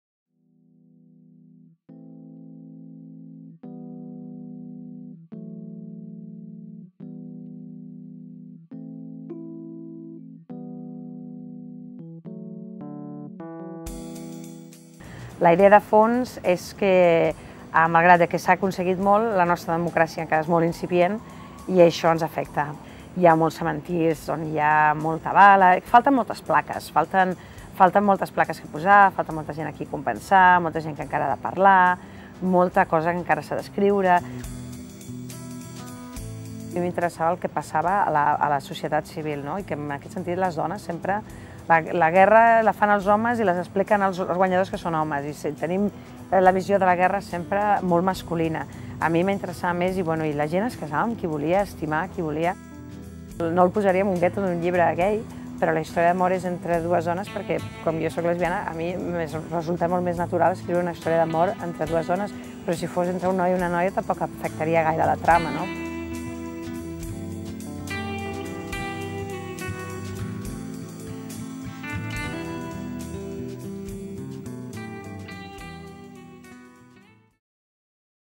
Elena Moya: "Encara falta molta gent per compensar per la Guerra Civil"
Estrevistem la periodista i escriptora que ha seduït el lector anglès amb 'Les oliveres de Belchite', una història de sentiments sobre la Guerra Civil Espanyola. + a